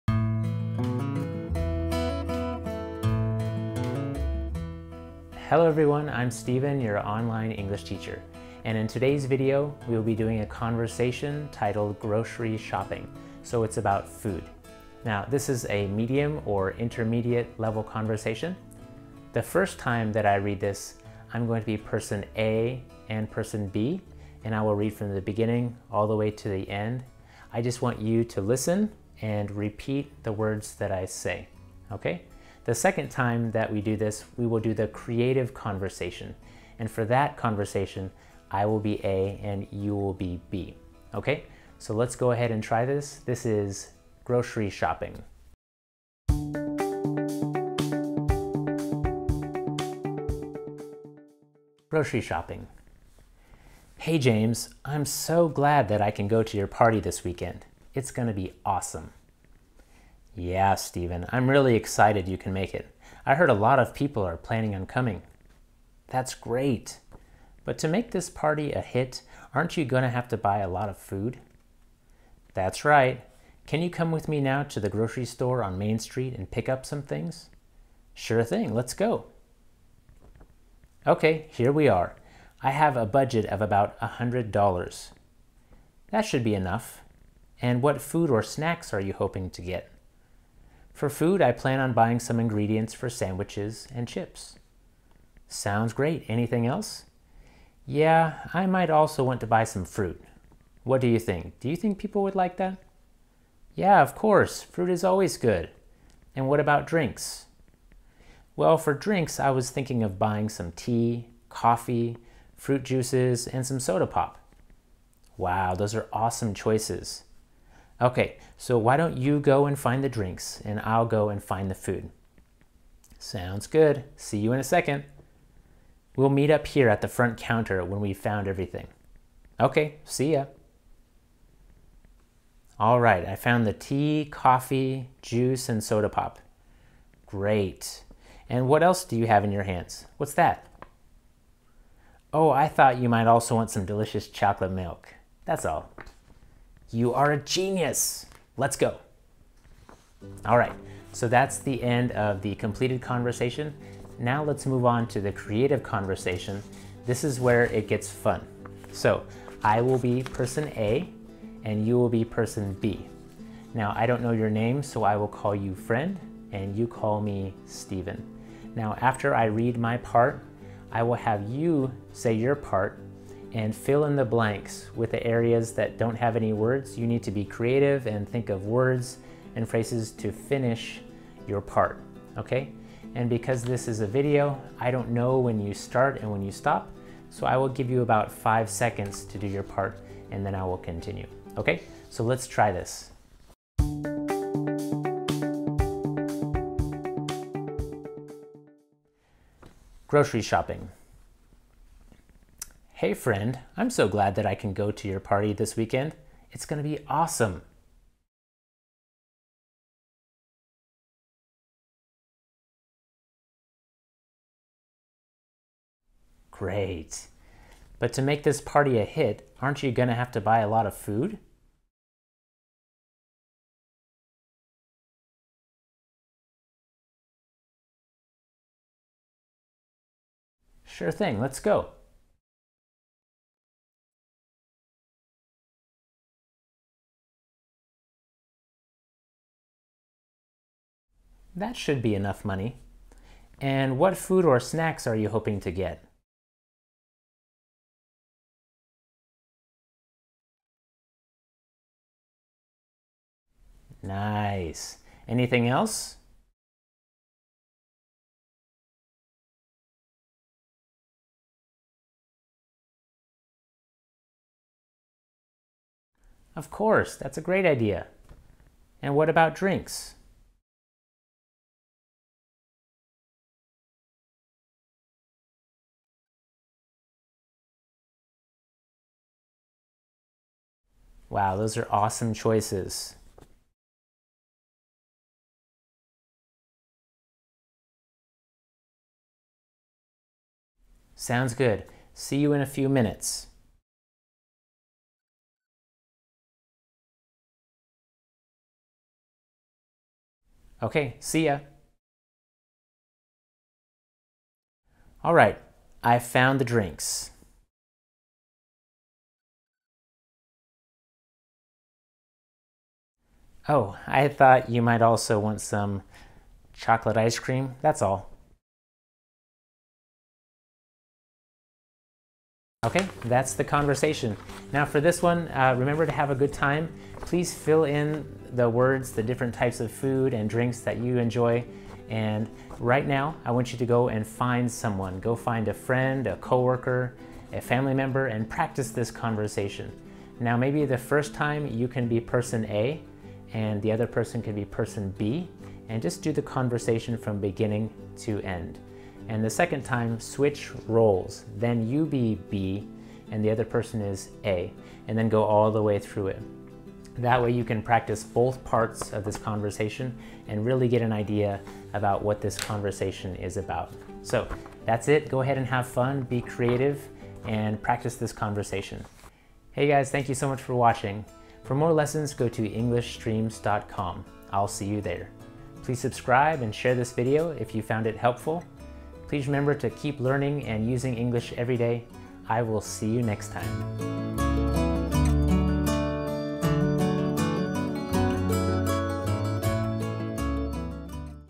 English Conversation: Grocery Shopping
This is a medium-level English conversation. Keep learning and using English everyday!